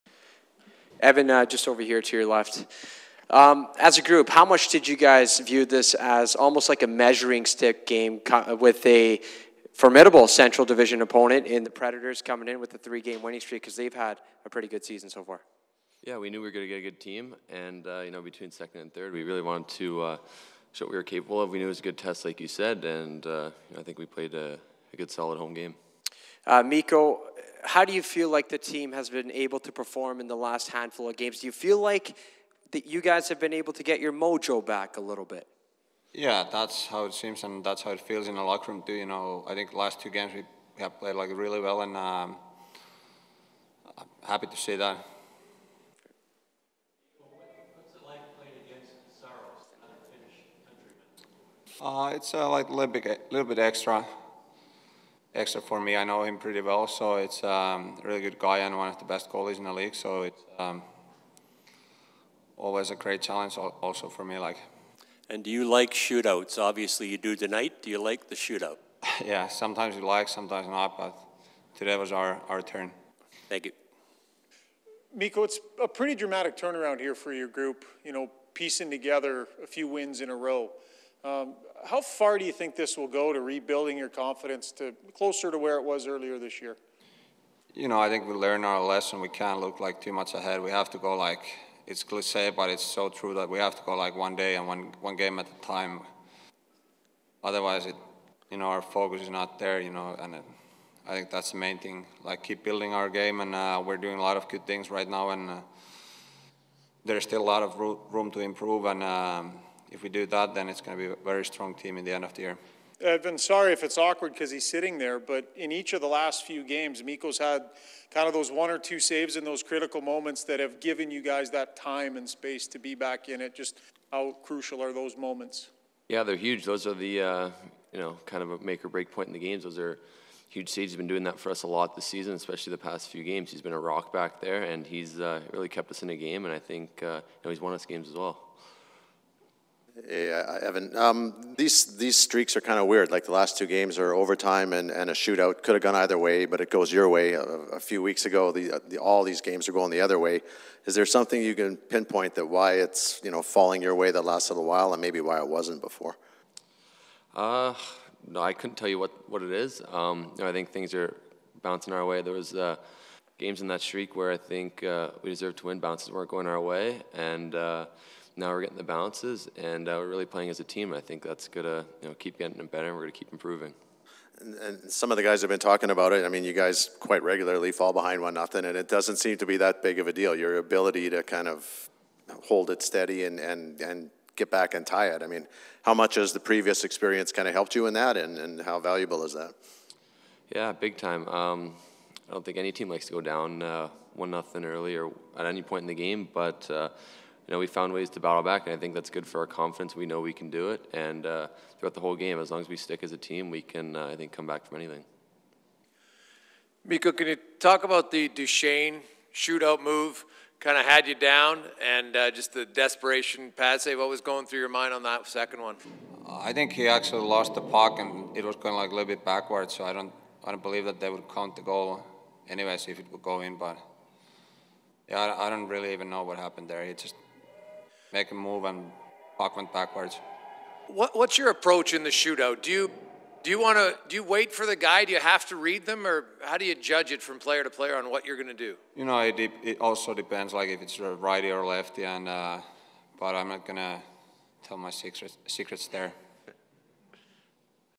POST-RAW | Evan Bouchard, Mikko Koskinen 01.27.22
Evan and Mikko take questions following the Oilers victory over the Predators Thursday night in Edmonton.